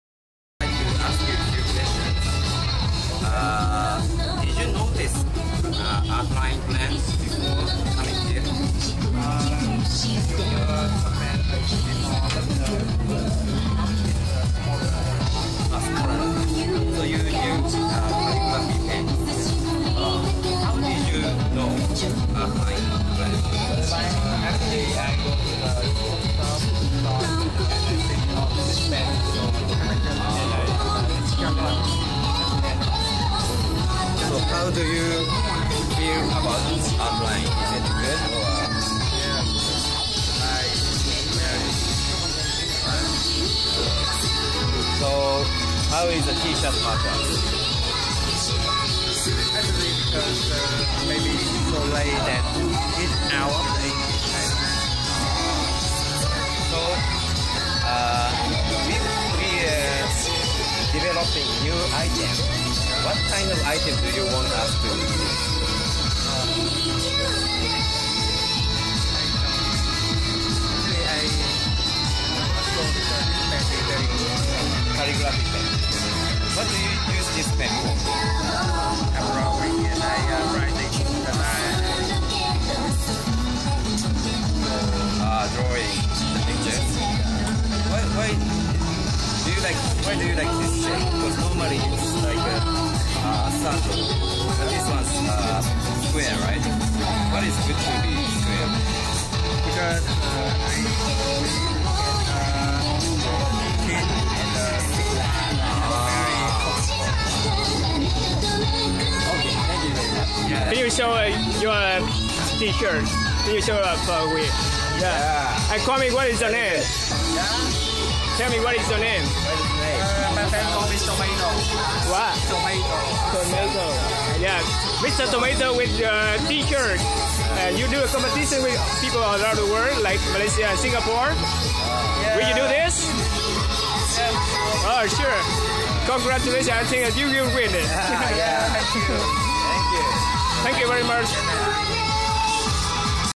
Phỏng vấn Bút Artline tại hội chợ sách lần 7
Cuộc thi vẽ trên áo tại hội chợ sách Tp.HCM lần 7 (Công Viên Lê Văn Tám)
19/03/12 - 25/03/12

 Artline dòng sản phẩm bút viết cao cấp của Nhật Bản
không chứa Xylene độc hại và đạt chuẩn RoHS của châu âu về độ an toàn với người tiêu dùng.